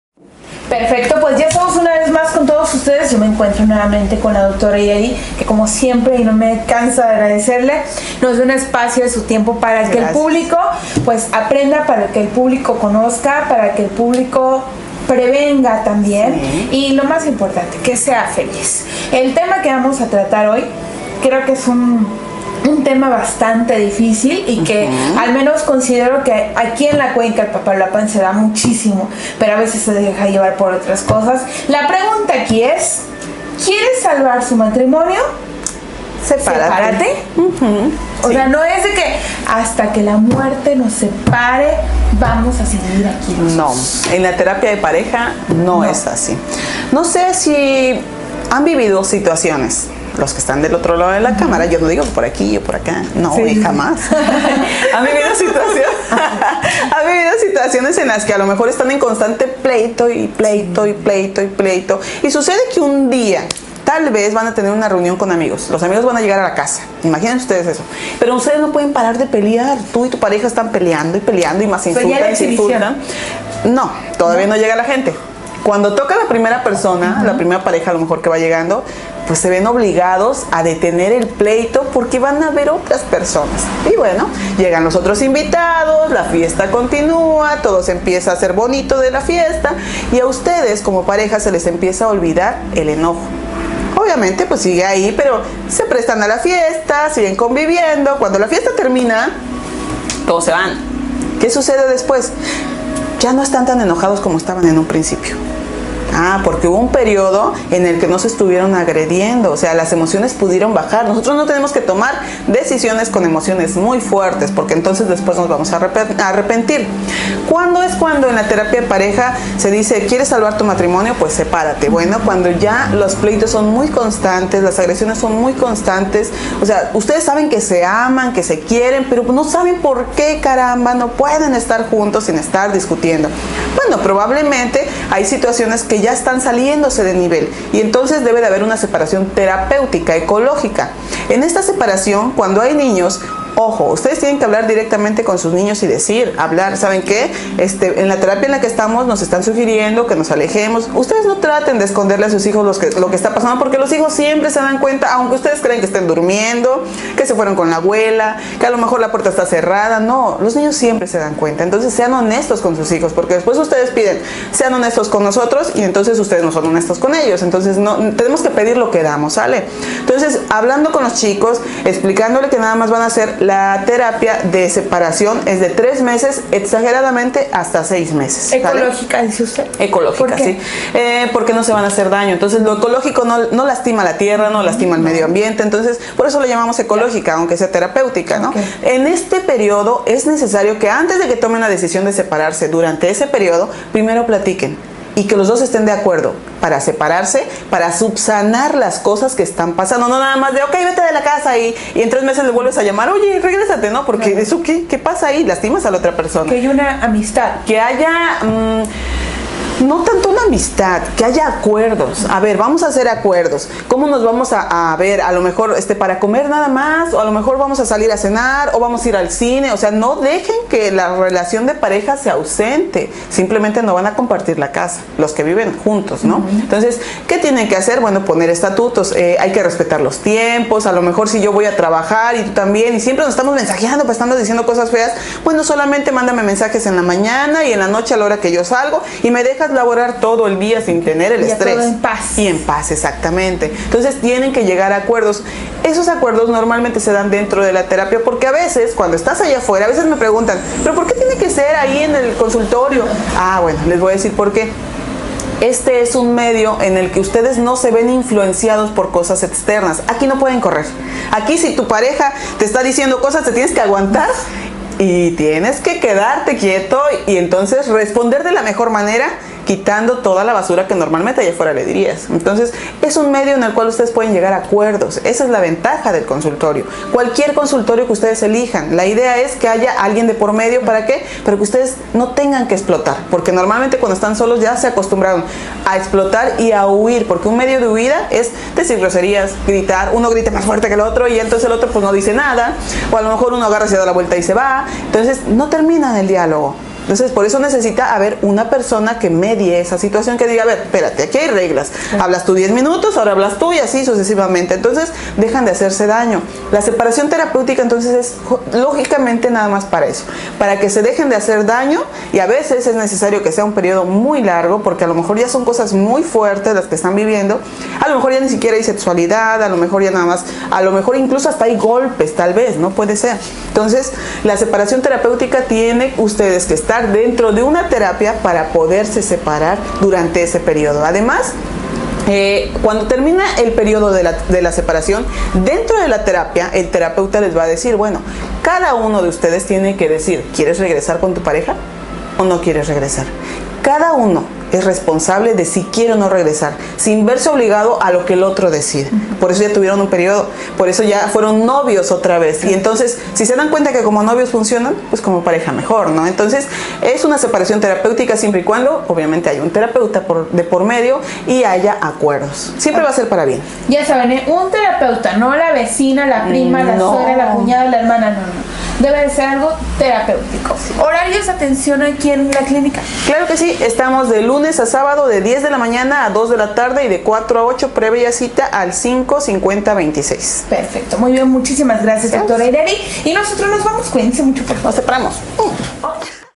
Dra Hirery - Separación Temporal
Es una opción cuando la tensión acumulada hace que la convivencia de pareja sea insostenible